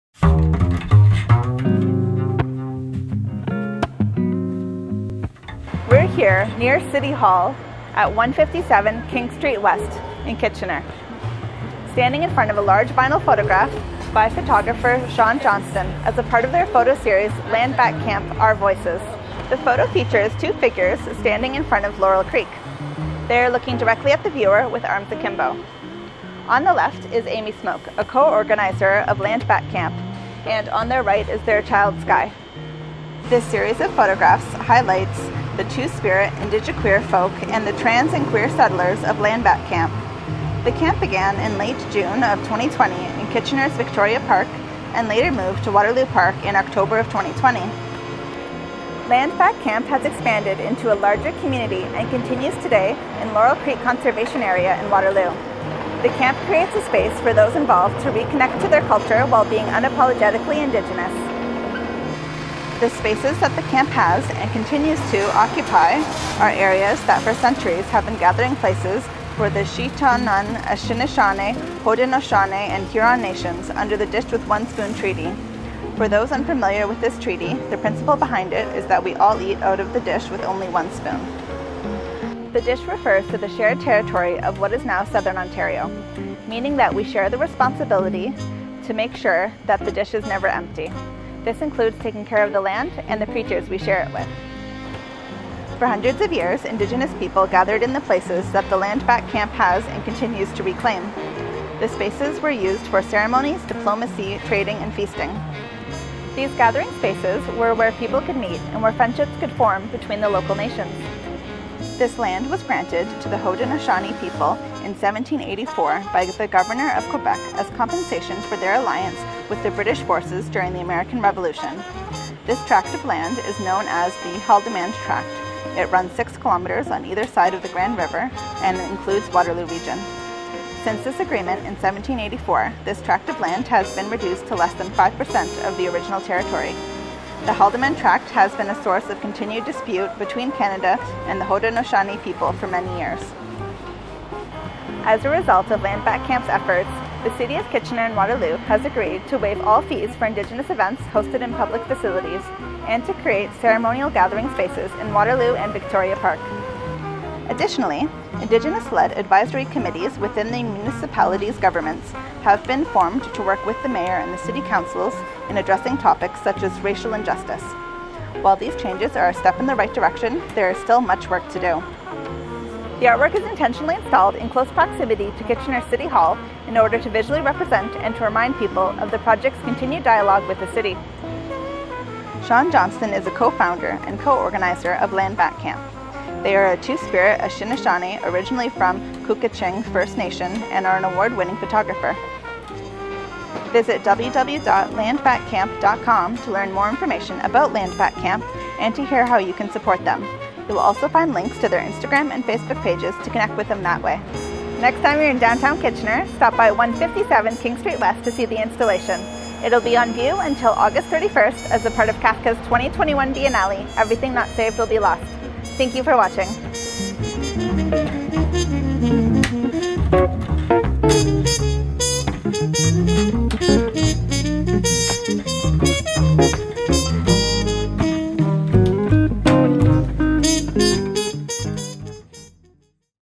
CAFKA.21: Shawn Johnston, Land Back Camp: Our Voices | Educational Virtual Tour
Shawn Johnston
“Land Back Camp: Our Voices”
Presented as part of the CAFKA.21 Biennial, "Everything Not Saved Will Be Lost"
On view until August 31, 2021 at 157 King St. W, Kitchener

“Land Back Camp: Our Voices” is a series of photos that spotlights the Two Spirit IndigiQueer folx and queer settlers of O:se Kenhionhata:tie, also known as Land Back Camp. The camp began in late June 2020 with only a teepee and two tents beside a playground in Kitchener’s Victoria Park, and later moved to Waterloo Park in Waterloo in October 2020. Its original intent was to temporarily “occupy” a space in the park – an area which had for centuries been a gathering place for Chonnonton, Anishinaabe, Haudenosaunee, and Huron Nations under the Dish With One Spoon Wampum. Land Back Camp continues today and are reclaiming space in Laurel Creek Conservation Area.

Shawn Johnston is a co-founder/ co-organizer of O:se Kenhionhata:tie, also known as Land Back Camp. They are a Two Spirit Anishinaabe originally from Couchiching First Nation and are an award-winning photographer.

Written and Hosted by: Christina DiPaola
Shot and Edited by: David Botros
Images courtesy of the artist